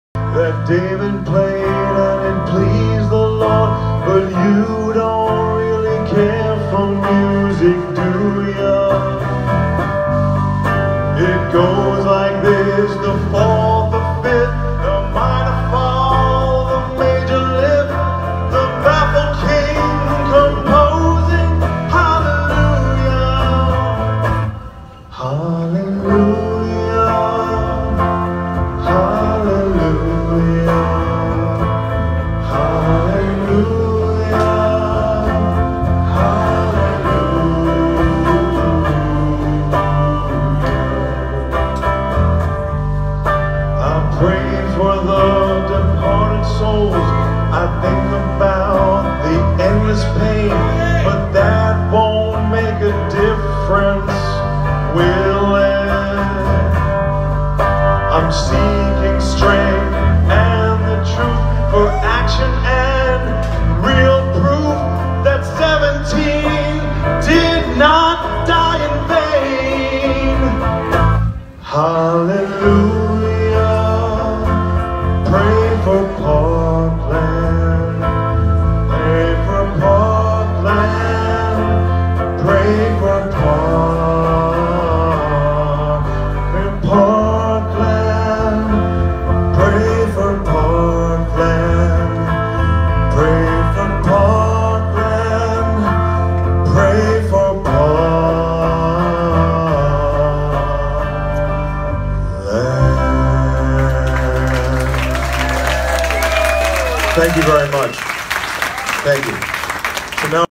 David Dweck honoring the Victims of the Parkland Marjory Stoneman Douglas High School Shooting
My good friend David Dweck honoring the Victims of the Parkland Marjory Stoneman Douglas High School Shooting